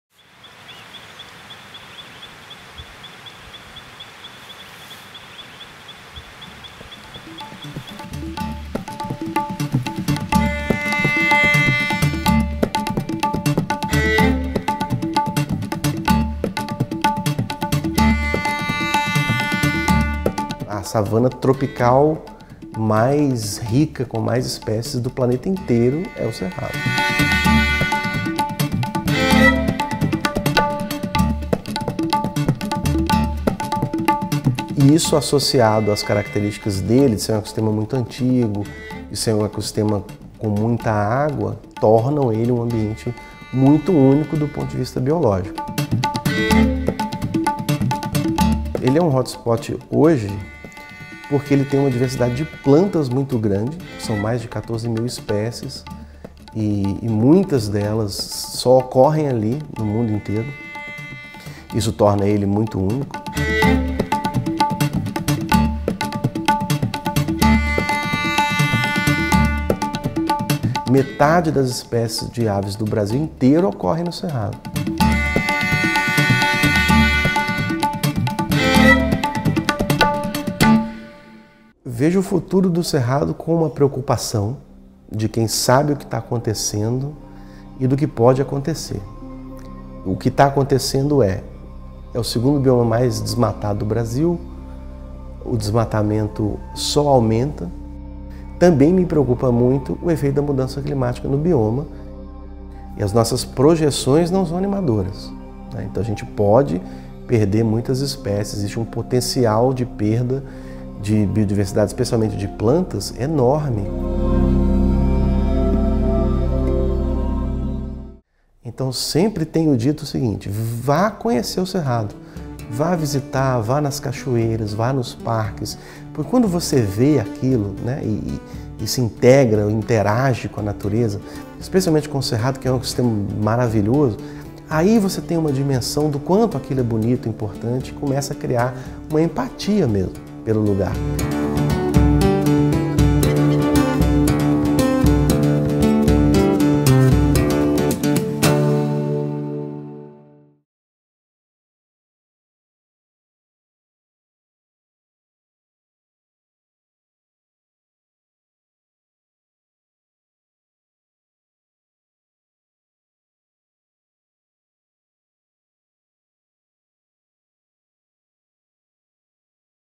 A vida do Cerrado é a nossa vida | Cerrado Vivo
A saúde do Cerrado é a saúde de todos nós. A vida do Cerrado é a vida de todos nós. Por isso, neste mês, em sintonia com o DiaNacionaldoCerrado (11 de setembro), o Museu do Amanhã dedicará conteúdos especiais para mostrar a importância de manter o CerradoVivo — acompanhe as nossas redes!